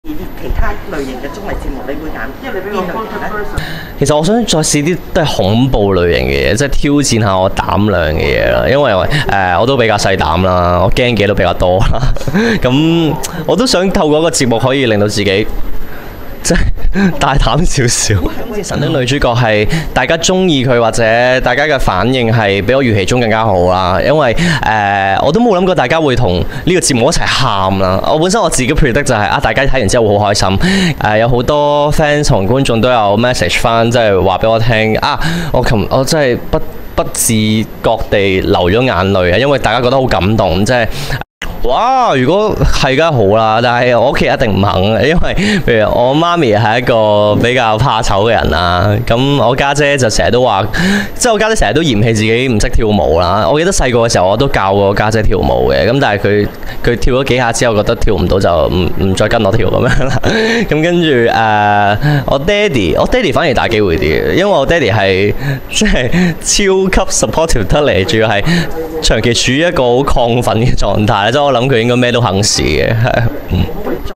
Anson Lo盧瀚霆想再做《入住請敲門》?
今年Anson Lo的綜藝節目《神的女主角》叫好叫座，曾經主持恐佈節目《入住請敲門》的他希望明年再有機會參與這類節目︰「我想再試恐佈類型節目，挑戰自己的膽量，我都比較細膽，驚的東西都比較多，我希望透過節目去令自己大膽少少！」問到會否考慮舉家一起參與綜藝節目，更能為自己和家人製造美好回憶；Anson Lo聞言興奮道：「嘩！可以的話當然好，但我估計家人應該未必肯，因為媽咪比較怕羞，家姐就經常嫌棄自己唔識跳舞，其實以前也有教過家姐跳舞，但她跳了幾下跟唔上就唔再學，唯獨爸爸可能機會較大，事關爸爸超級支持我之餘，還長期處於很亢奮狀態、所以估計他什麼都肯嘗試。」